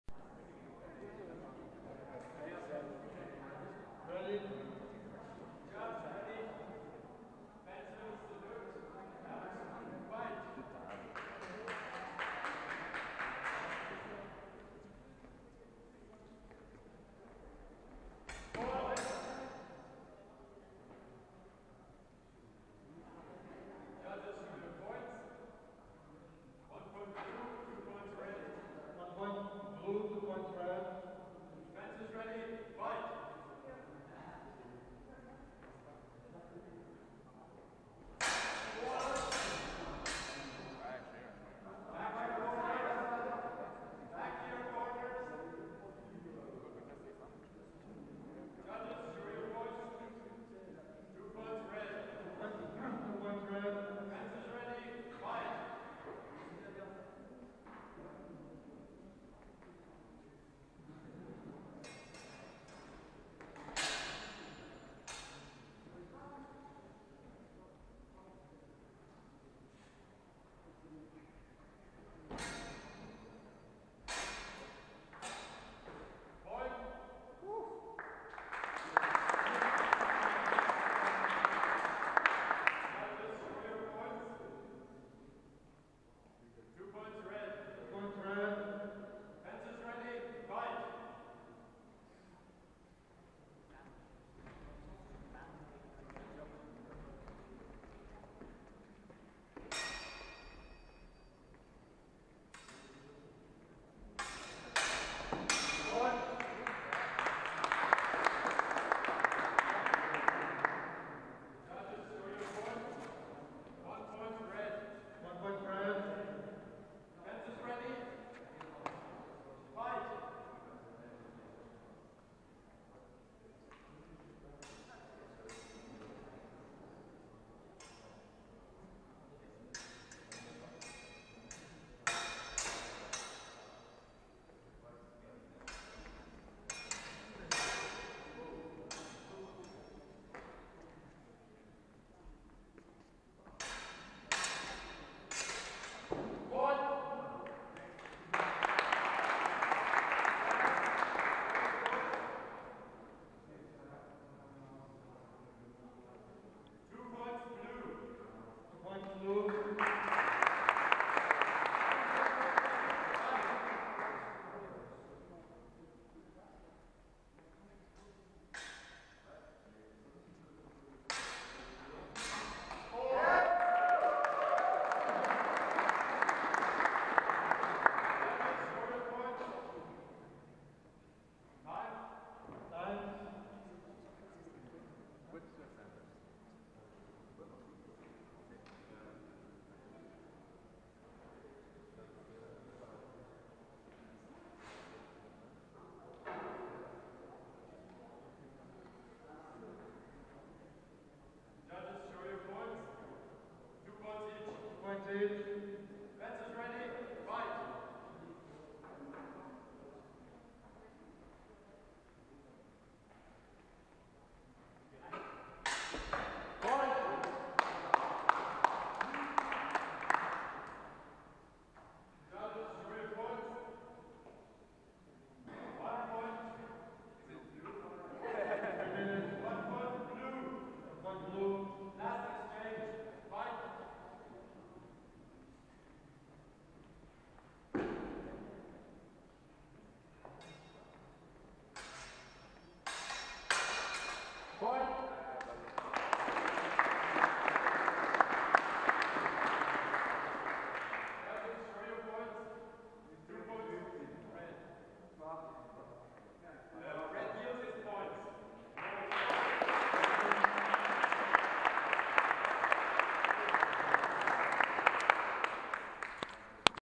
Swiss Bears Cup 2020 - Top 32 - Annabelle Luisa Conrady vs. Samuel Gutknecht
Annabelle Luisa Conrady (INDES, AUT, blue) vs. Samuel Gutknecht (Liberi Ensis, SUI, red)